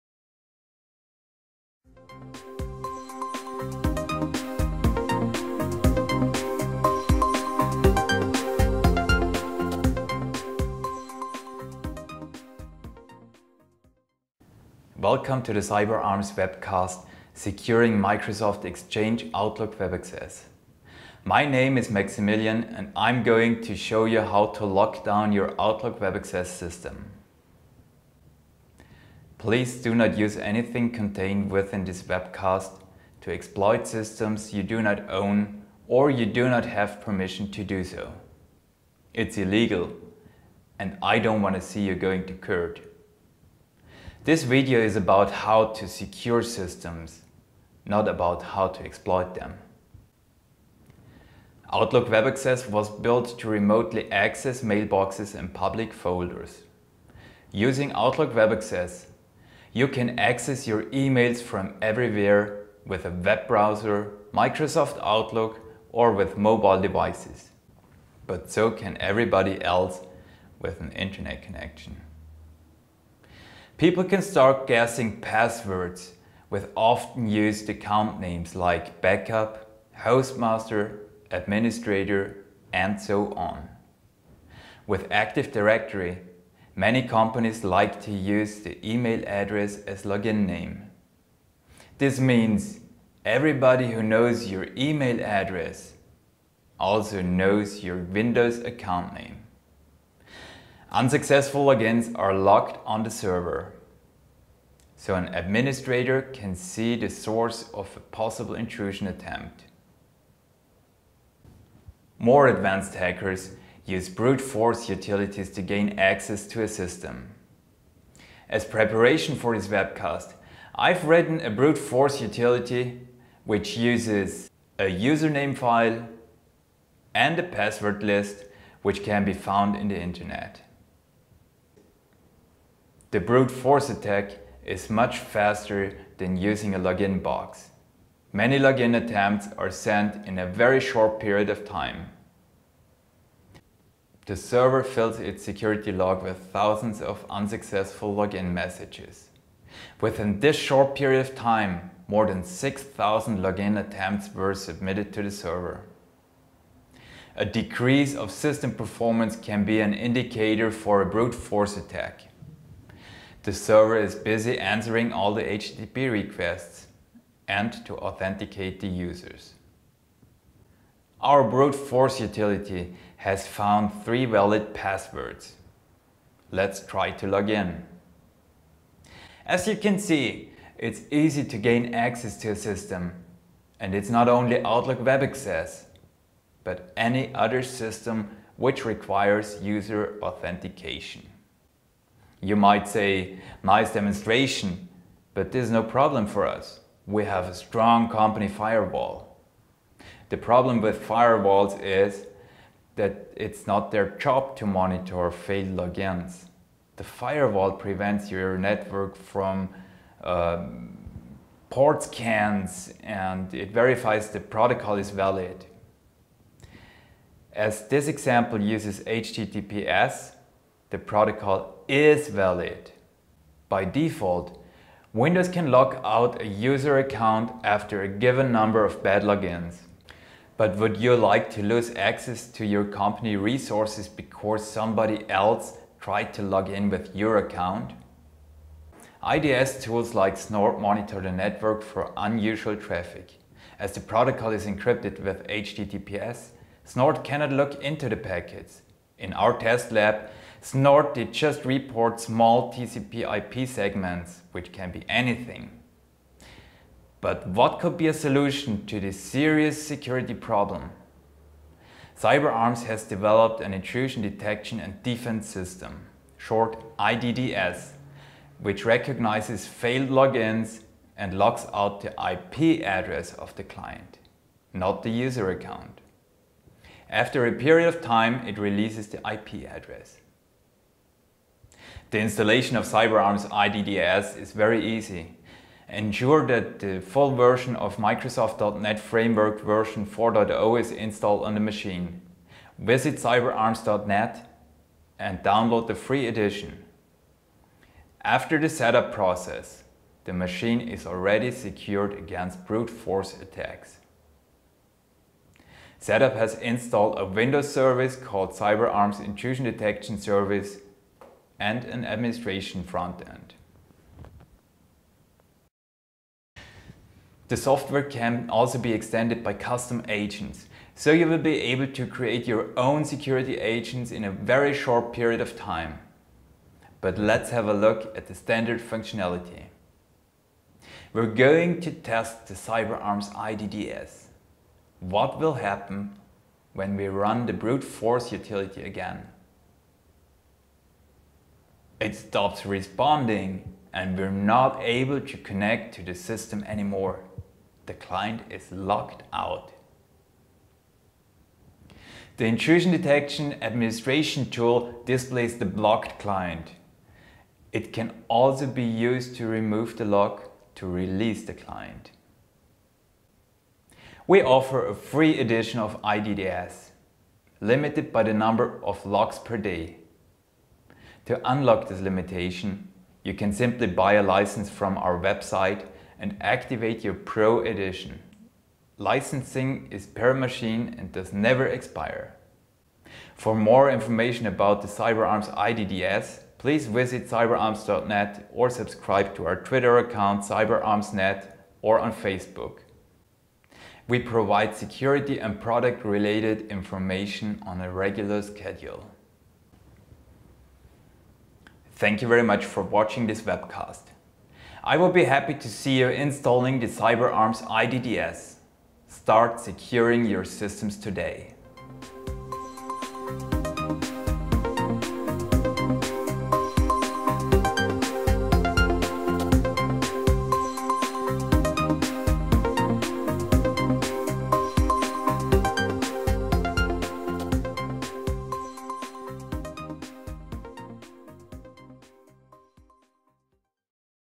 Securing Microsoft Exchange Outlook Web Access (OWA)
This webcast demonstrates how to lock down an Exchange Outlook Web Access Server with the Cyberarms Intrusion Detection and Defense System (IDDS). Brute force attacks happen on a daily basis. Keep your systems secure!